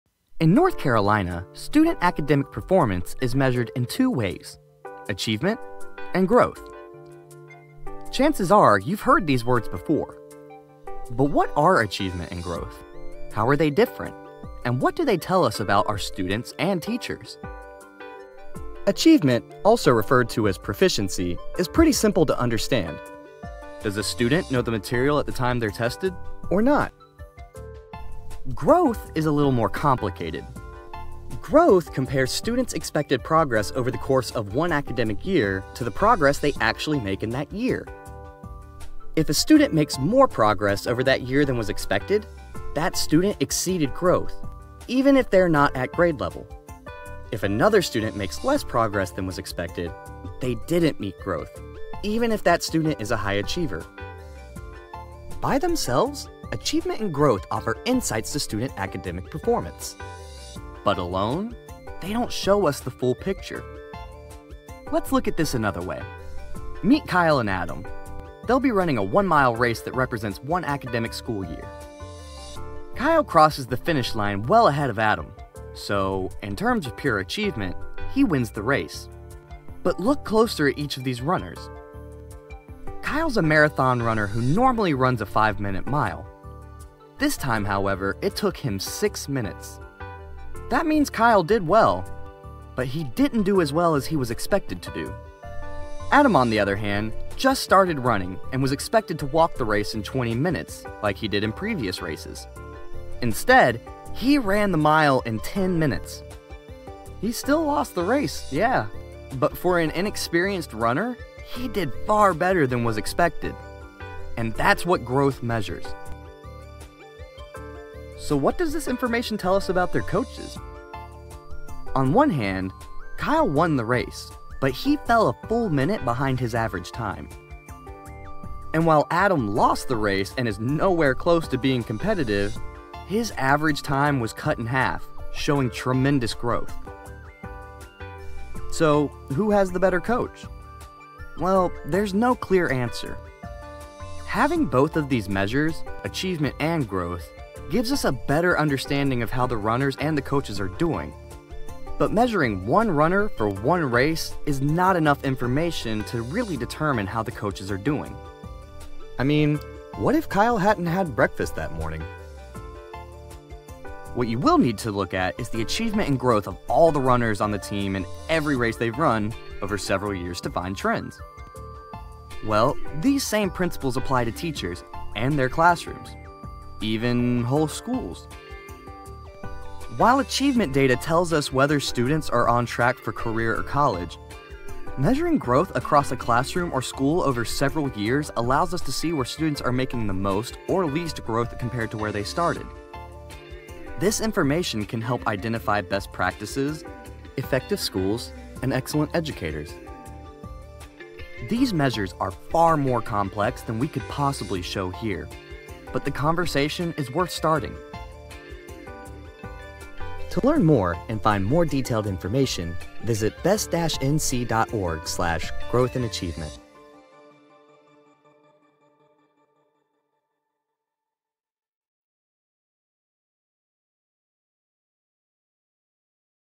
Growth and Achievement in North Carolina
Growth and achievement: you have probably heard these terms in conversations about education. But what are they? How do they differ, and what do they tell us about North Carolina's students and teachers?